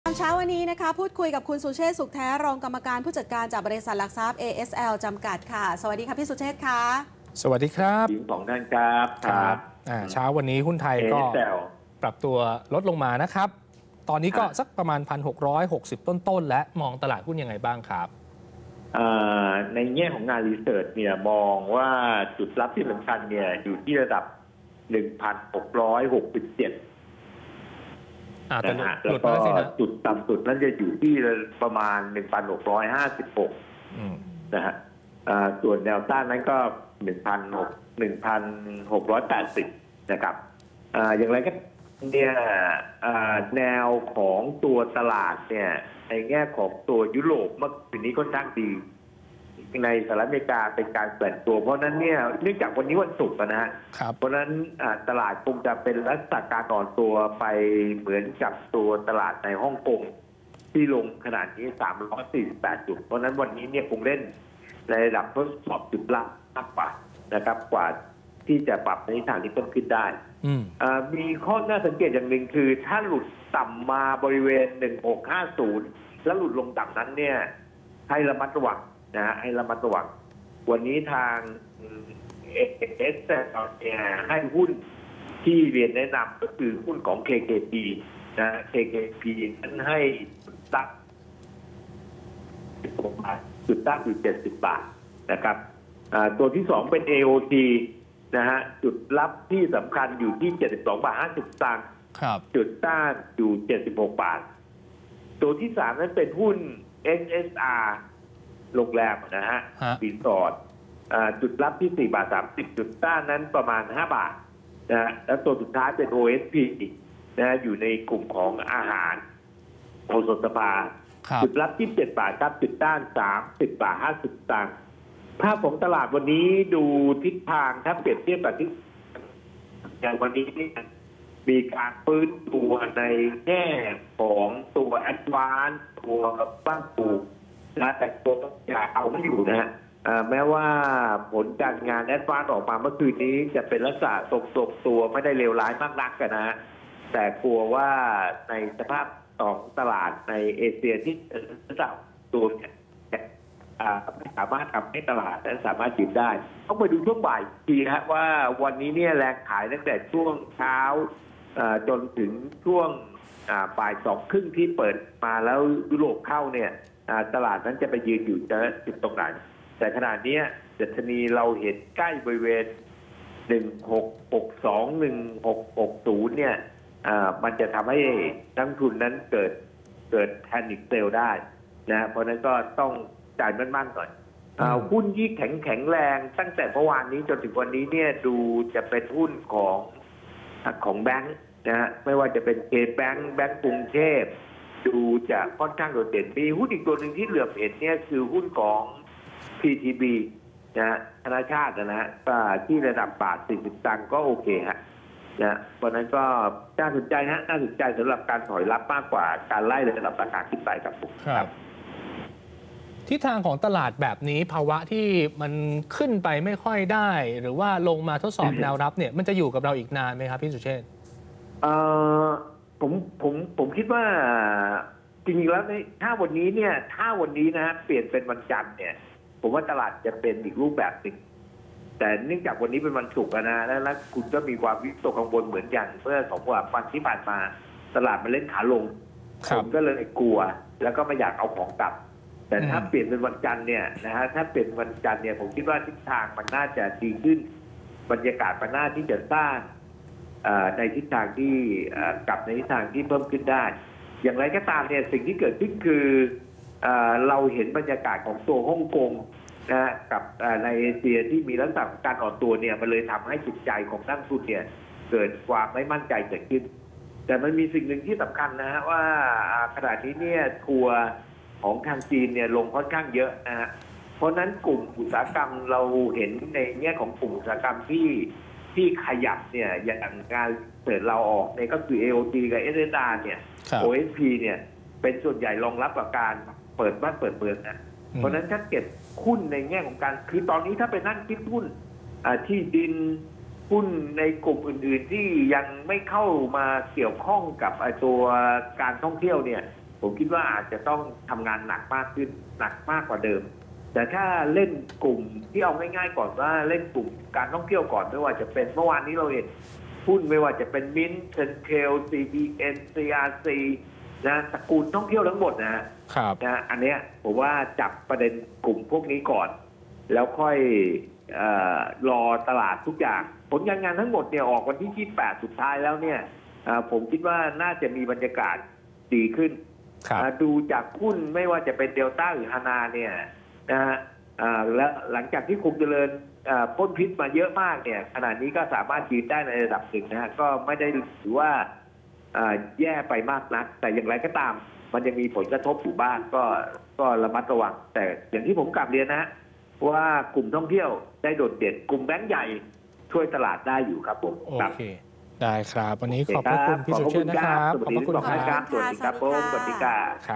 หุ้นไทย จุดรับสำคัญ 1,667 จุด หุ้นที่แข็งแรง คือ กลุ่มแบงก์
หุ้นไทย จุดรับสำคัญ 1,667 หุ้นที่แข็งแรงตั้งแต่เมื่อวาน จนถึงวันนี้ คือ กลุ่มแบงก์